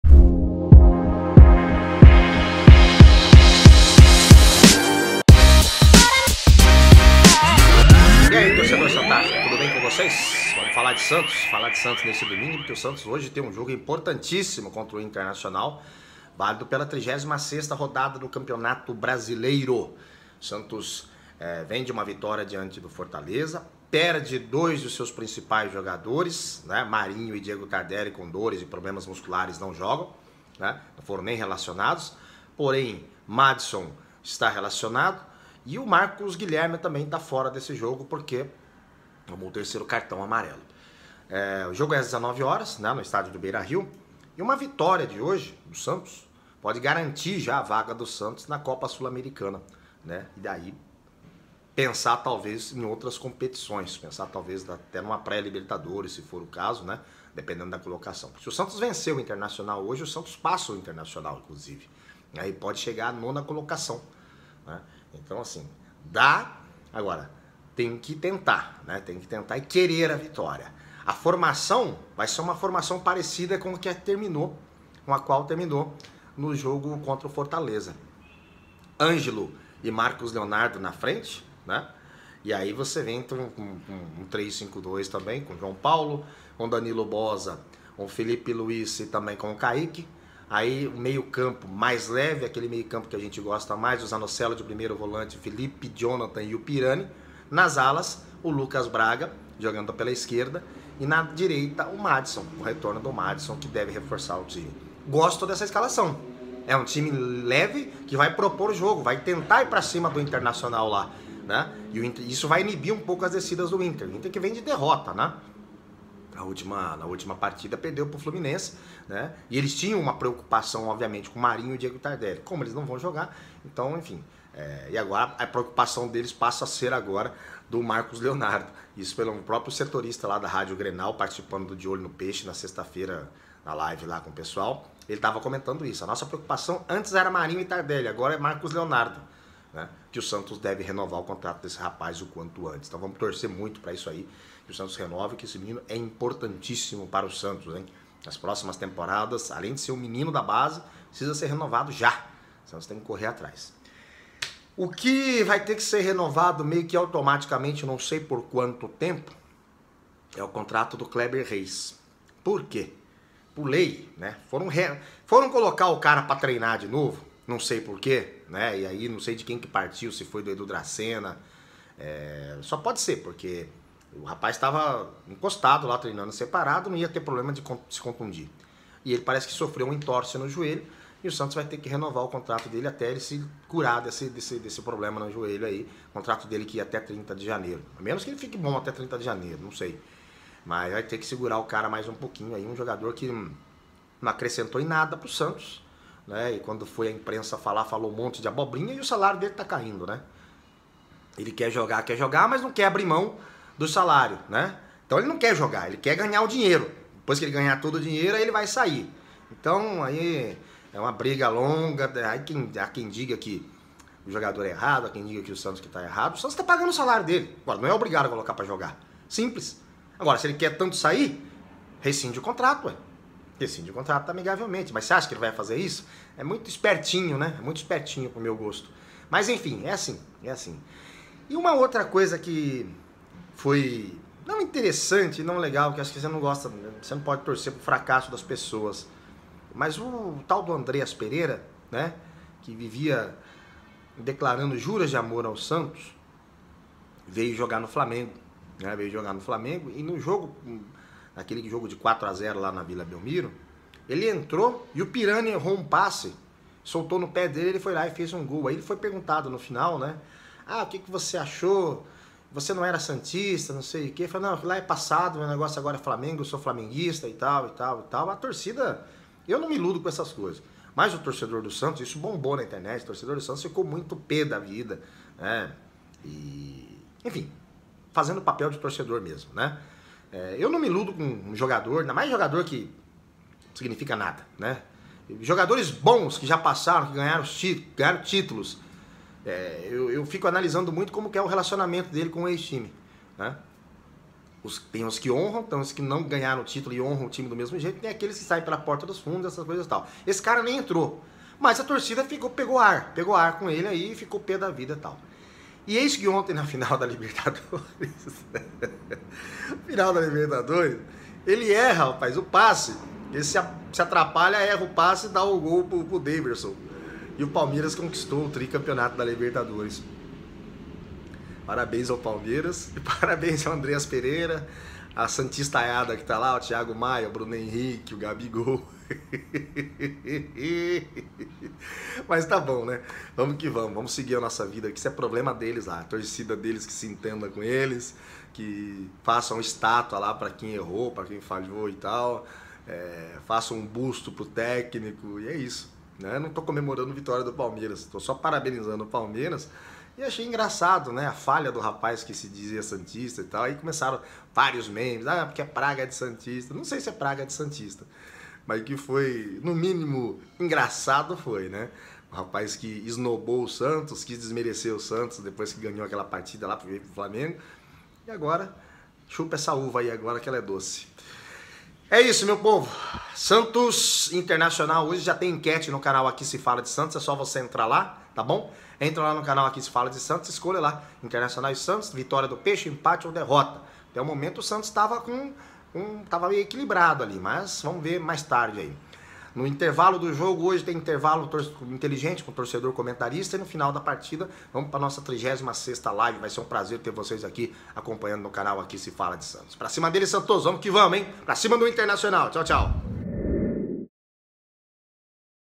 Santos vai de Marcos Leonardo e Ângelo para tentar bater o Internacional
Com Marcos Leonardo e Ângelo no ataque, o Santos vai enfrentar o Internacional neste domingo às 19h no Beira Rio em Porto Alegre. Marinho e Tardelli lesionados e Marcos Guilherme suspenso pelo terceiro cartão desfalcam o Santos. O técnico Fábio Carille deve manter o esquema 3-5-2 e tentar com essa formação, trazer os 3 pontos na bagagem.